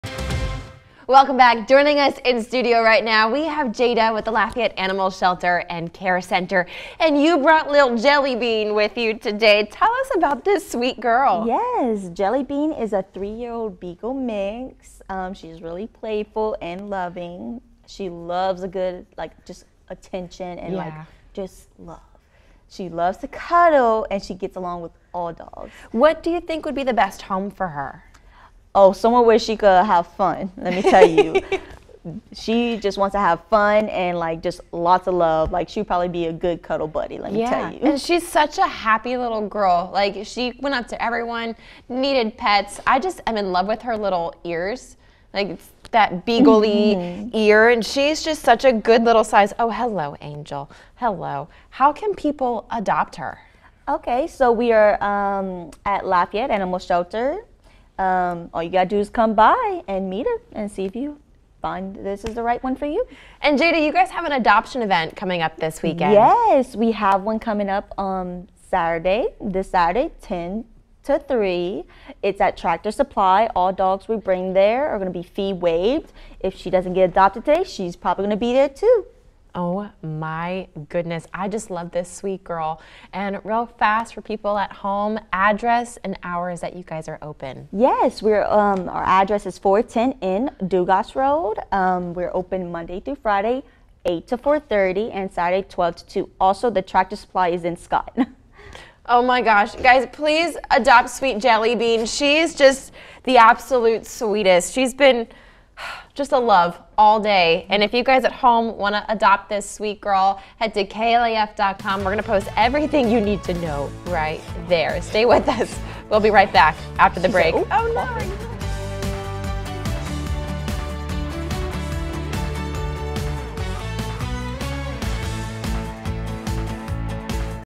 Furever Home Friday! Meet Jellybean, A Beagle Mix Looking For A New Home
Jada Duhon, Adoption Coordinator with Lafayette Animal Shelter and Care Center, joined News15 at Noon to help find Jellybean her "furever home". Jellybean is a 3 year old beagle mix who loves attention, scratches and lots of couch cuddles.